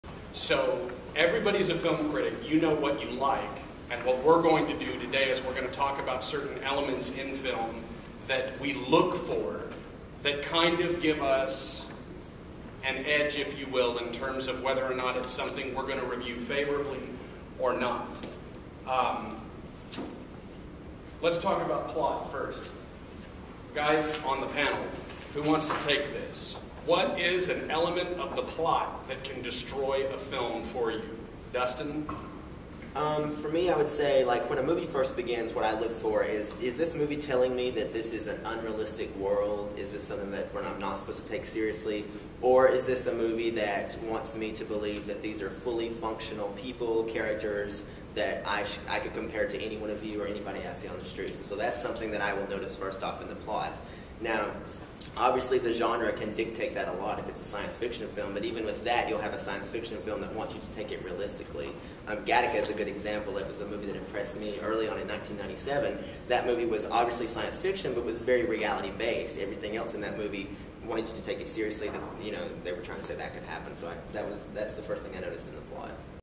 Houston Film Critics Society panel discussion at 2011 World Fest film festival Houston Texas
Five members from HFCS discuss what they look for in the plot of a film when they review movies. Texas Art & Film critic Dustin Chase talks about setting the viewer up for a realistic story versus something that is unrealistic.